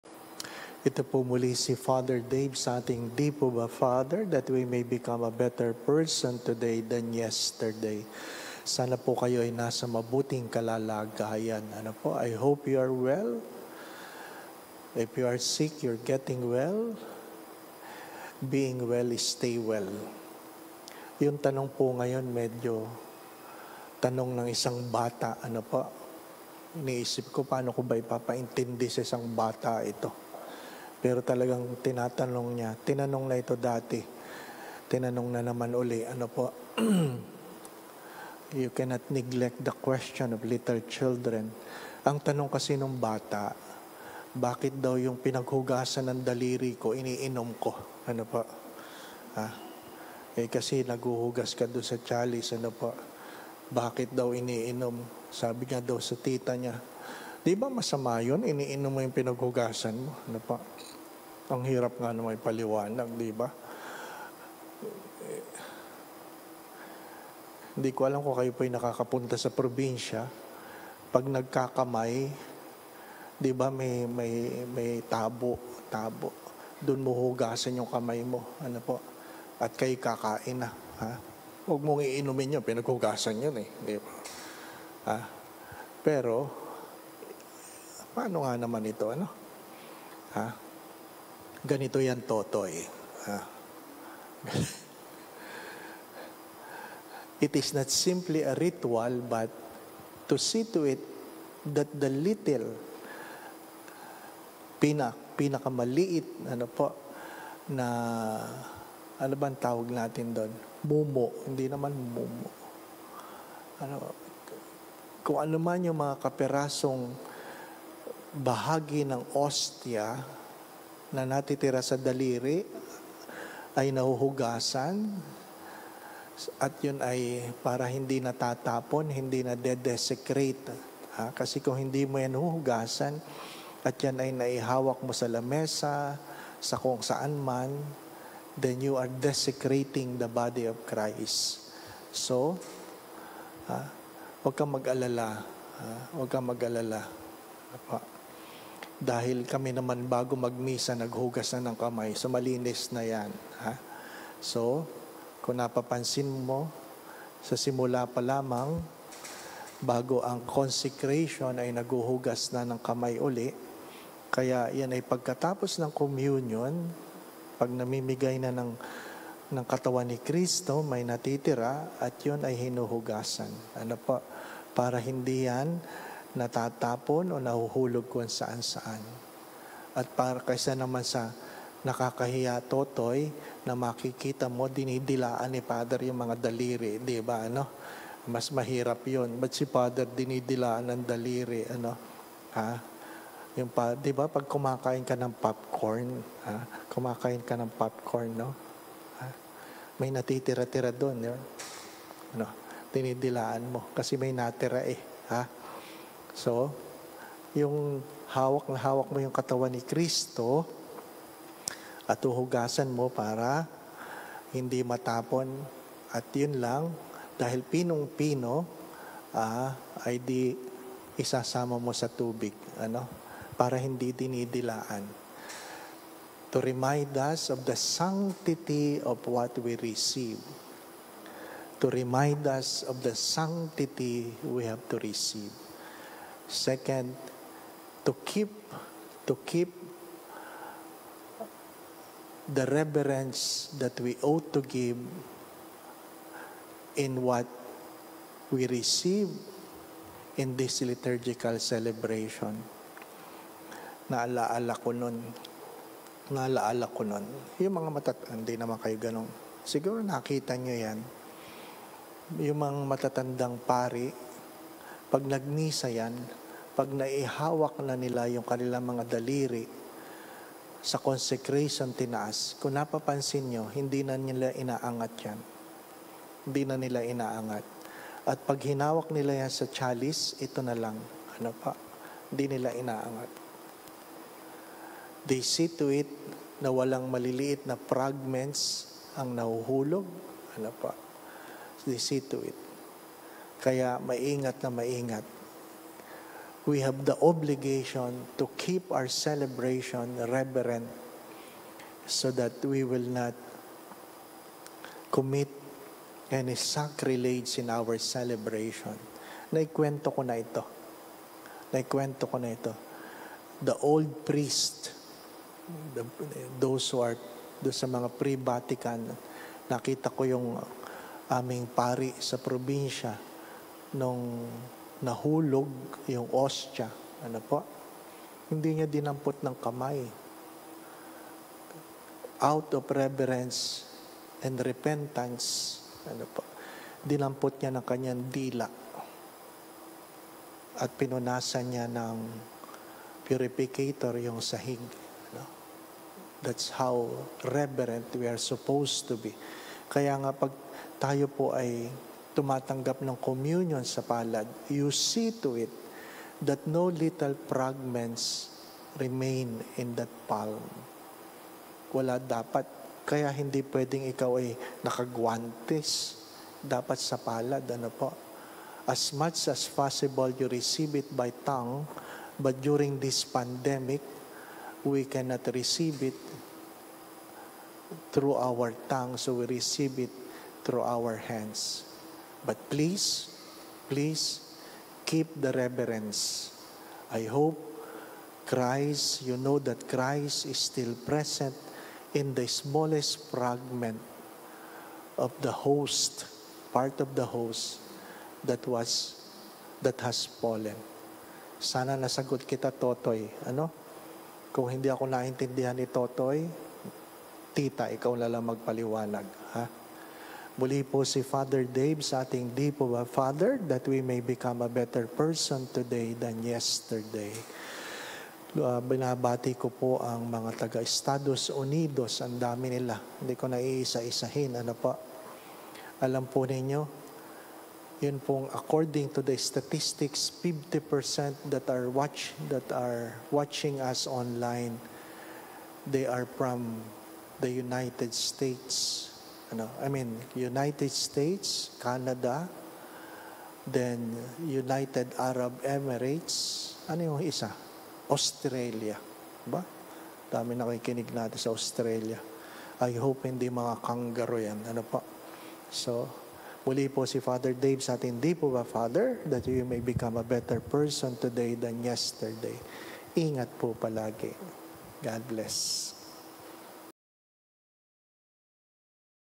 dipobafrdave (Ep. 207) - BAKIT INIINOM ANG PINAGHUGASAN NG DALIRI?
'DI PO BA, FR. DAVE? Q & A with Fr. Dave Concepcion after the 7:00am weekday mass at Sta. Maria Goretti Parish (March 6, 2021)